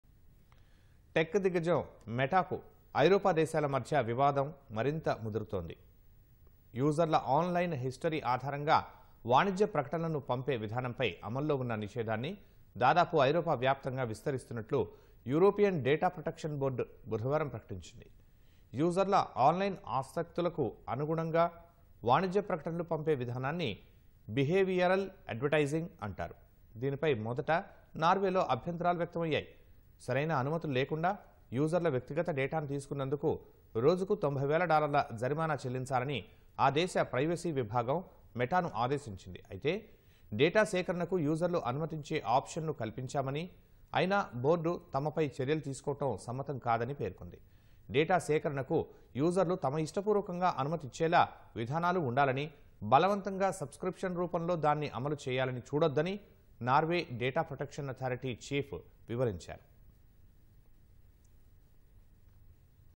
Disputes Between European Countries to Meta | Ban On Mode of Sending Meta’s Commercials in Europe
టెక్ దిగ్గజం మెటాకు ఐరోపా దేశాల మధ్య వివాదం మరింత ముదురుతుంది. యూజర్ల ఆన్ లైన్ హిస్టరీ ఆధారంగా వాణిజ్య ప్రకటనలను పంపే విధానంపై అమల్లో ఉన్న నిషేధాన్ని... దాదాపు ఐరోపా వ్యాప్తంగా విస్తరిస్తున్నట్లు యురోపియన్ డేటా ప్రొటెక్షన్ బోర్డ్ ’బుధవారం ప్రకటించింది. యూజర్ల ఆన్ లైన్ ఆసక్తులకు అనుగుణంగా వాణిజ్య ప్రకటనలు పంపే విధానాన్ని బిహేవియరల్ అడ్వర్టయిజింగ్ అంటారు. దీనిపై మొదట నార్వేలో అభ్యంతరాలు వ్యక్తమయ్యాయి. సరైన అనుమతులు లేకుండా యూజర్ల వ్యక్తిగత డేటాను తీసుకున్నందుకు...రోజుకు 90 వేల డాలర్ల జరిమానా చెల్లించాలని ఆ దేశ ప్రైవసీ విభాగం మెటాను ఆదేశించింది. అయితే, డేటా సేకరణకు యూజర్లు అనుమతించే ఆప్షన్ ను కల్పించామని. అయినా బోర్డు తమపై చర్యలు తీసుకోవడం సమ్మతంకాదని పేర్కొంది. డేటా సేకరణకు యూజర్లు తమ ఇష్టపూర్వకంగా అనుమతిచ్చేలా విధానాలు ఉండాలని...బలవంతంగా సబ్ స్క్రిప్షన్ రూపంలో దాన్ని అమలు చేయాలని చూడొద్దని... నార్వే డేటా ప్రొటెక్షన్ అథారిటీ చీఫ్ వివరించారు